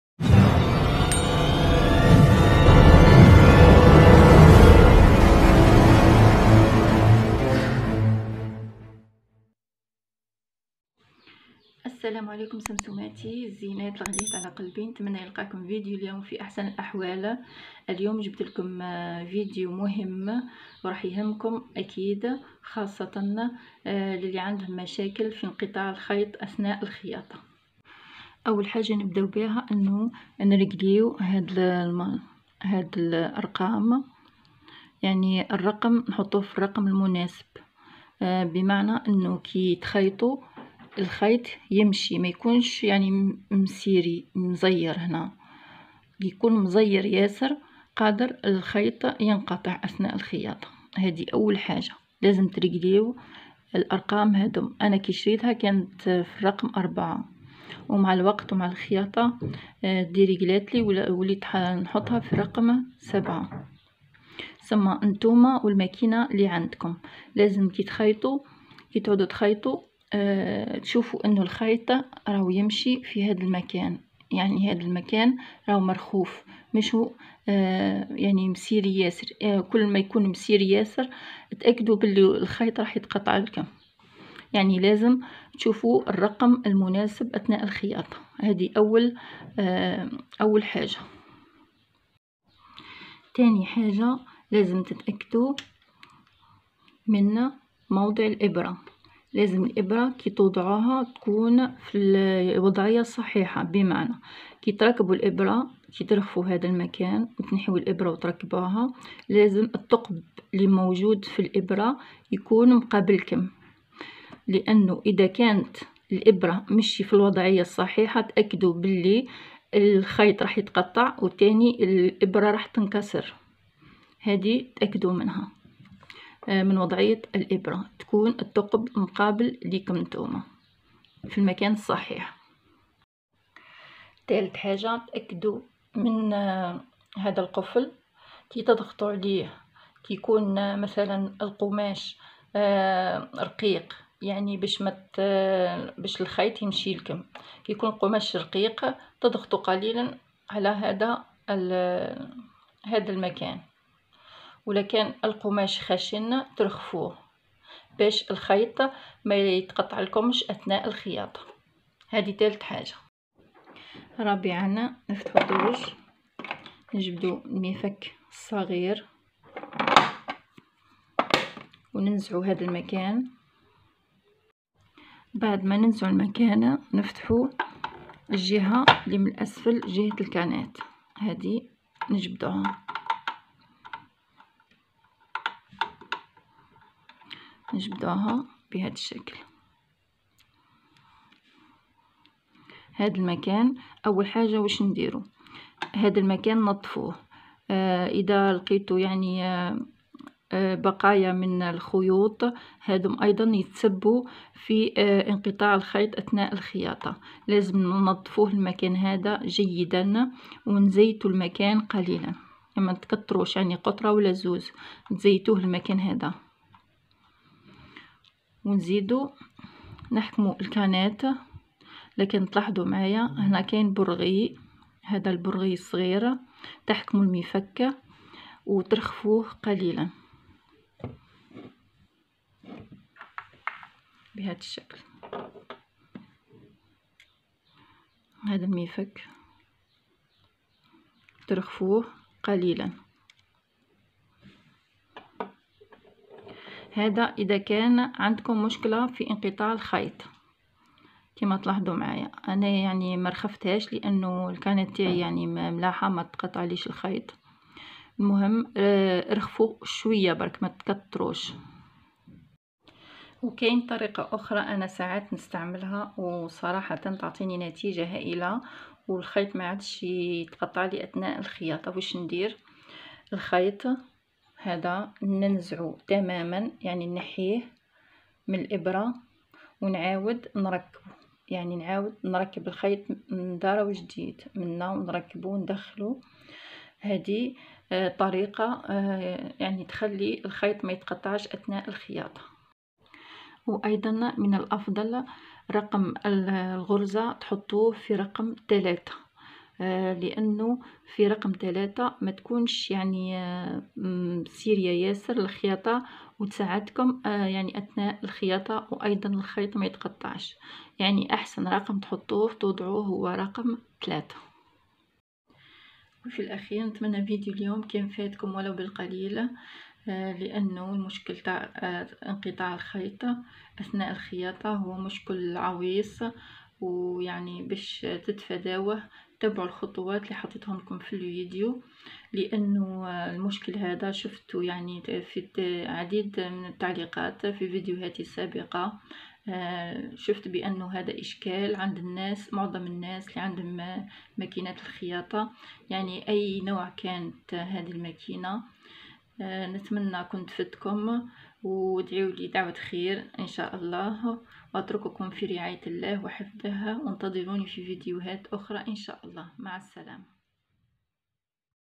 حل مشكلة تقطع الخيط بماكينة سوبر زيڨزاڨ Super Zig Zag
حلول أسباب تقطع الخيط أثناء الخياطة بمكينة سوبر زيڨزاڨ

حل مشكلة تقطع الخيط بمكينة سوبر زقزاق, super Zig Zag, تقطع الخيط, أسباب تقطع الخيط أثناء الخياطة,خياطة,مكينة.